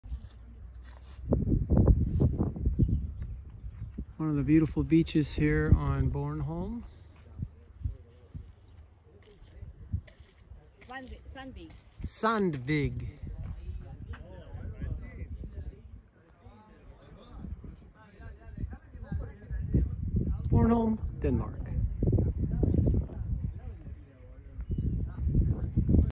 Bornholm, Denmark. Ahhh summer at the beach! Baltic Sea style!
A Baltic Sea paradise! The island of Bornholm,Denmark. the beach of Sandvig!